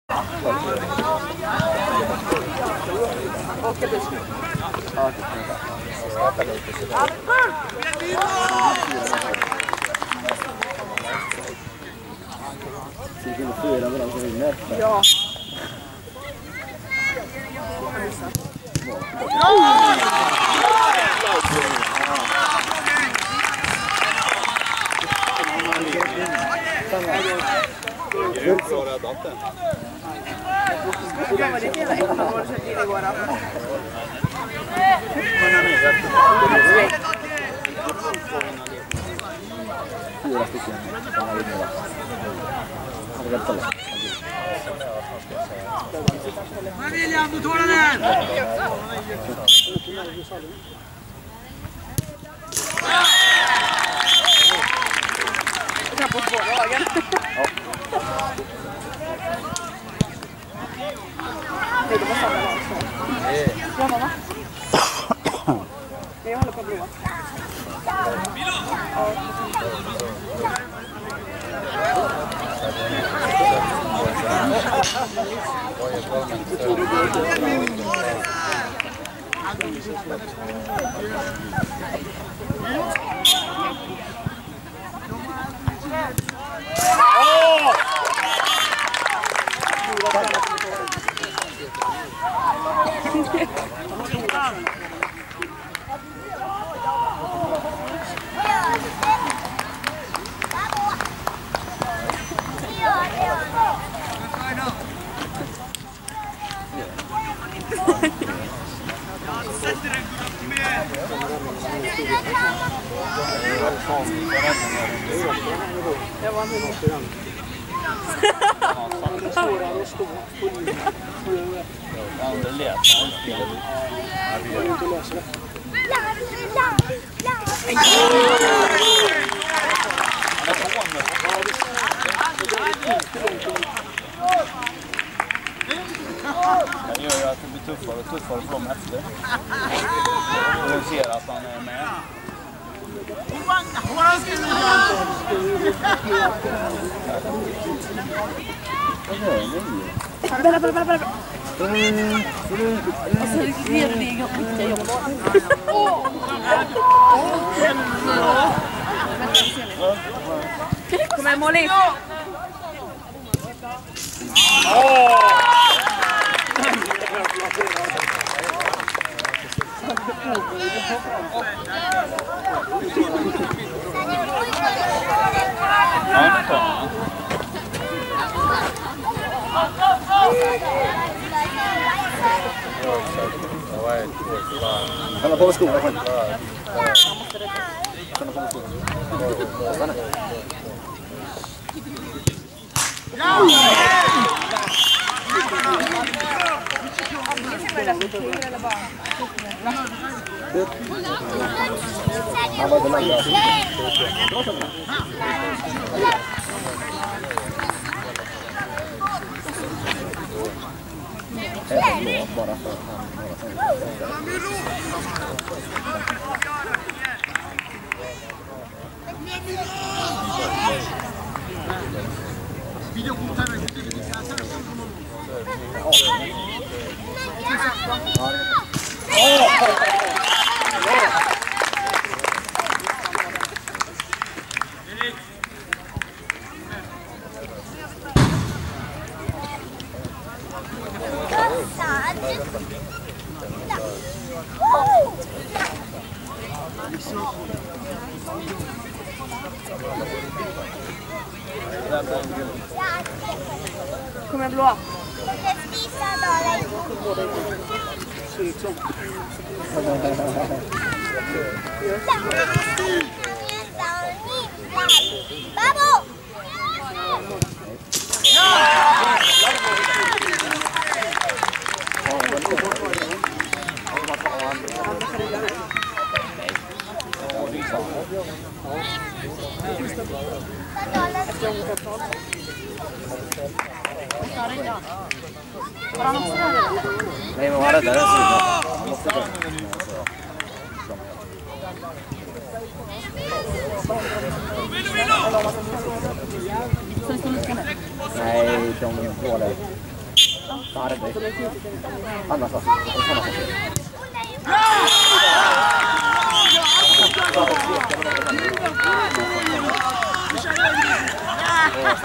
Gothia Cup: Spårvägens FF - FBK Karlstad (Straffläggning)
Gothic Cup 2016
Boys 16
Resultat: 5 - 4 (efter straffar)